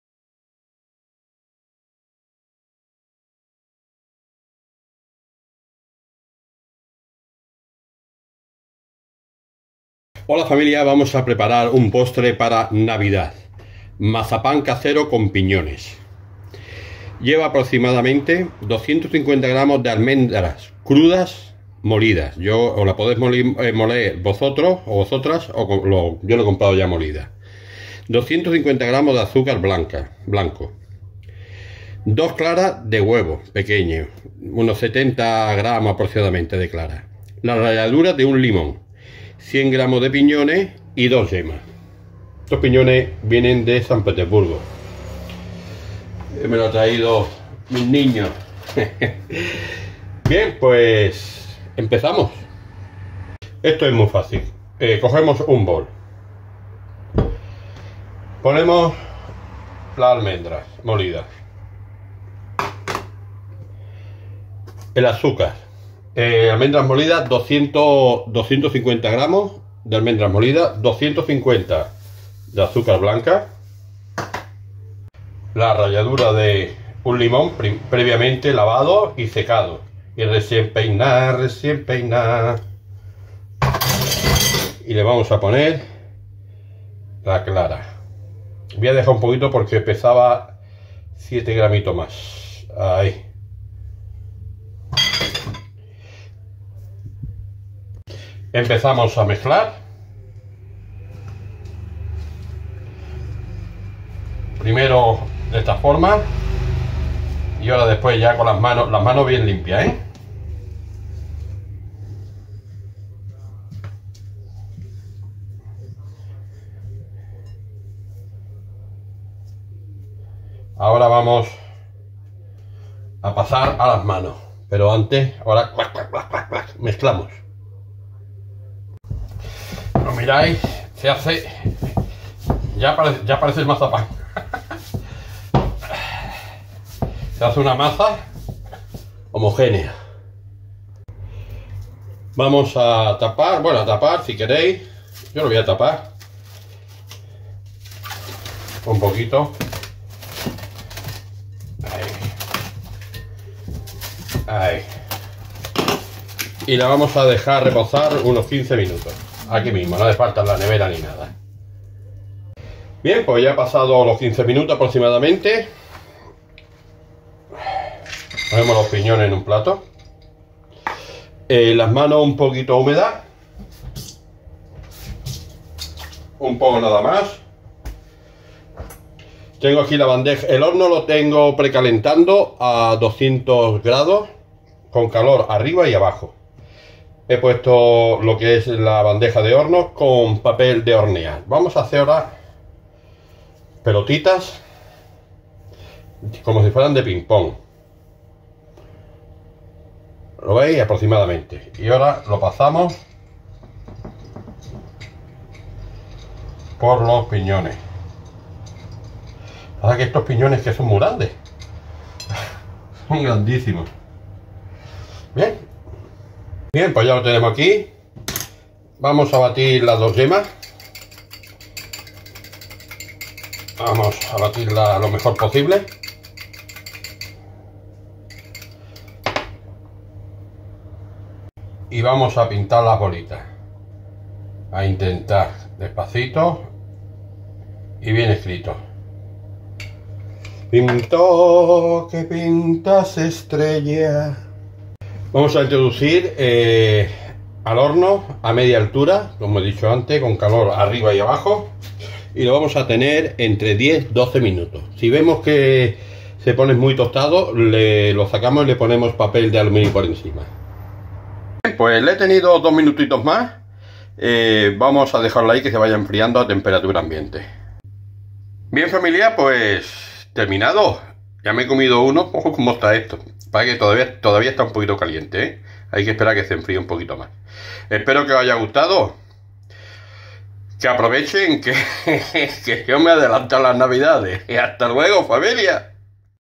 Mazapán casero con piñones: Homemade marzipan with pine nuts.Christmas.
Mazapán casero con piñones. Navidad.
Ingredientes:
250 gr. de almendra cruda (entera o molida). 250 gr. de azúcar blanco. 2 claras de huevos pequeños (70 gr.). Ralladura de 1 limón. 100 gr. de piñones. 2 yemas.

Preparación:

Molemos la almendra. La pasamos a un bol grande. Le añadimos el azúcar, las claras de huevo, la ralladura de limón y vamos mezclando/amasando. Primero con un tenedor y luego con las manos. Obtendremos una masa compacta, ligeramente húmeda. La dejamos reposar 15 minutos. Humedecemos las manos. Vamos haciendo bolitas con las manos, de un tamaño como pelotas de ping-pong. Rebozamos en piñones, cubriendo toda la superficie. Precalentamos el horno: 10 minutos a 200º C. Colocamos papel sulfurizado en la bandeja del horno. Vamos colocando los dulces separados entre sí. Los pintamos con yema de huevo (la sobrante del principio). Colocamos la bandeja en posición central y horneamos 10-12 minutos, a 200º C. Dependiendo de la potencia de vuestro horno el tiempo puede variar. Si veis que se tuestan demasiado, colocad un papel aluminio sobre ellos. Retiramos la bandeja y los dejamos enfriar, así cogerán la consistencia necesaria.

Homemade marzipan with pine nuts.Christmas.
Ingredients:
250 gr. raw almonds (whole or ground). 250 gr. of white sugar. 2 small egg whites (70 gr.). Zest of 1 lemon. 100 gr. of pine nuts. 2 yolks.
Preparation:
We grind the almond. We put it in a large bowl. We add the sugar, the egg whites, the lemon zest and we mix/knead. First with a fork and then with your hands. We will obtain a compact, slightly moist dough. We let it rest for 15 minutes. We moisten our hands. We make balls with our hands, the size of ping-pong balls. We coated in pine nuts, covering the entire surface. Preheat the oven: 10 minutes at 200º C. Place parchment paper on the oven tray. We are placing the sweets separately from each other. We paint them with egg yolk (the leftover from the beginning). We place the tray in a central position and bake for 10-12 minutes, at 200º C. Depending on the power of your oven, the time may vary. If you see that they are toasting too much, place an aluminum foil over them. We remove the tray and let them cool, this way they will reach the necessary consistency.